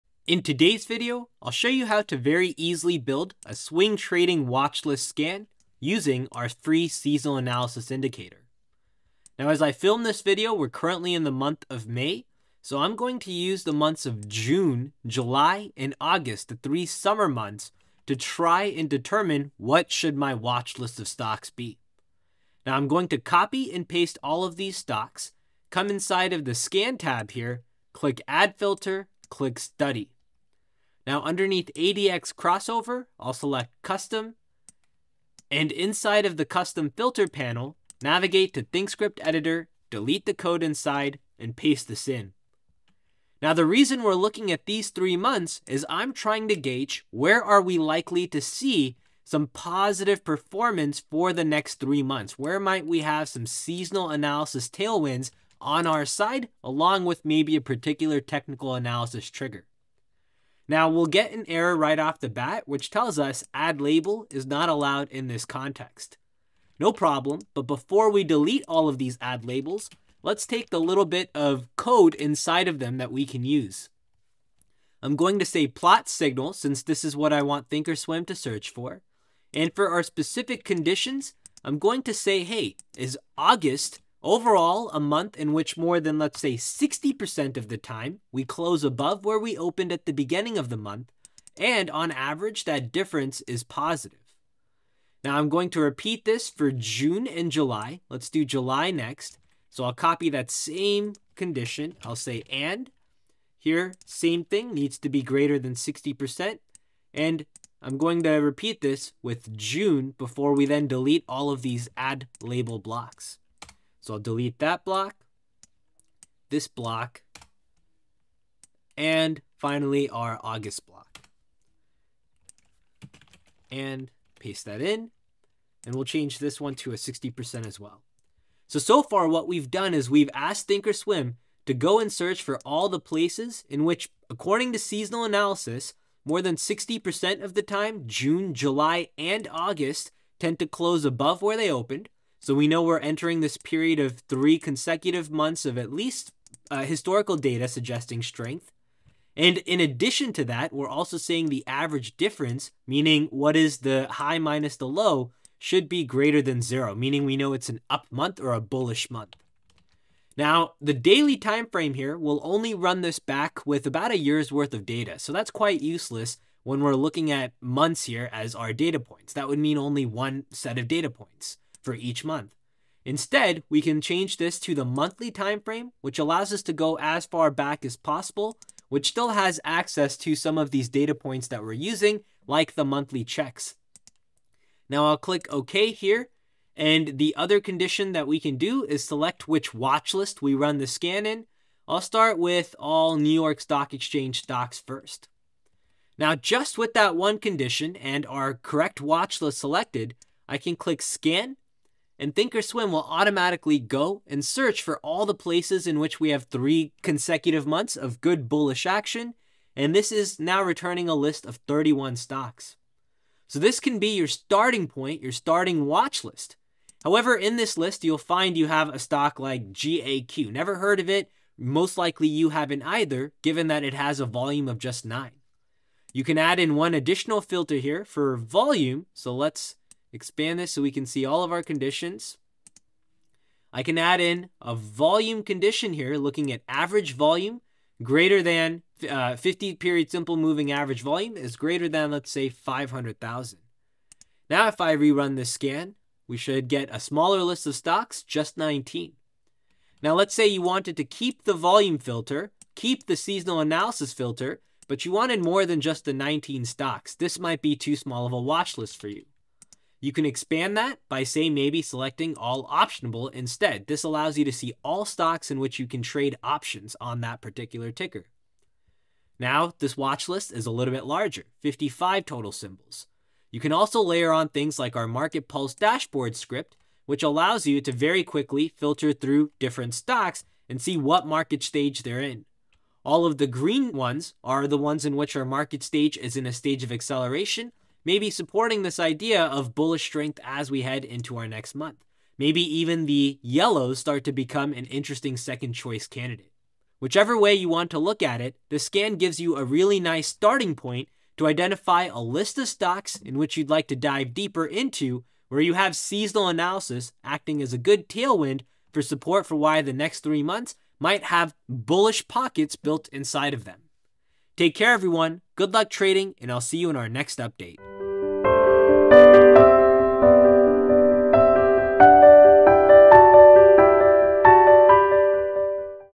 Swing Trading Scan for Strong Historical Performance
Free ThinkorSwim scan to find stocks with strong historical performance for the upcoming 3-month period.

⏱️ Here are video timestamps that might come in handy:

0:00 - ThinkOrSwim Scan Overview
0:25 - Building the Scan
3:15 - Demo
3:40 - Volume Filter
4:30 - Summary